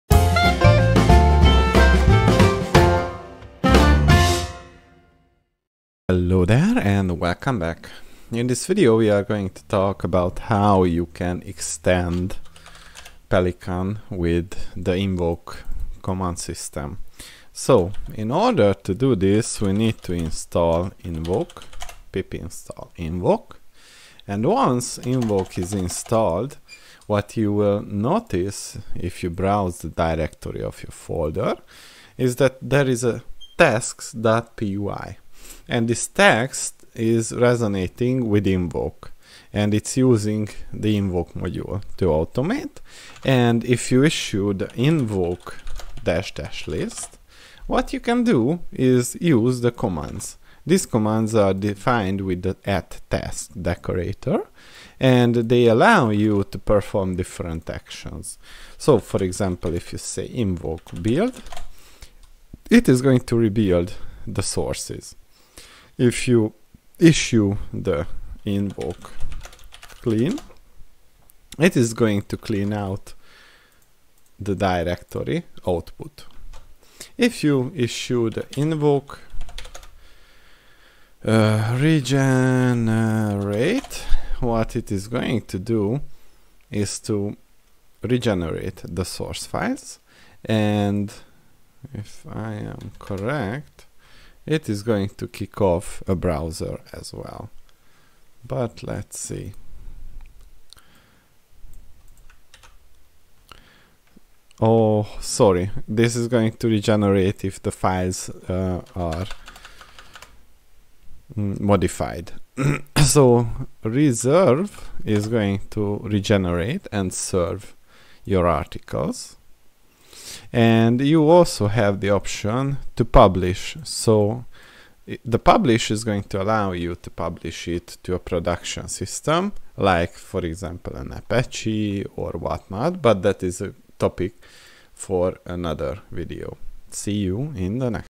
Python - Pelican invoke
In this video you will learn how to use the invoke feature in pelican.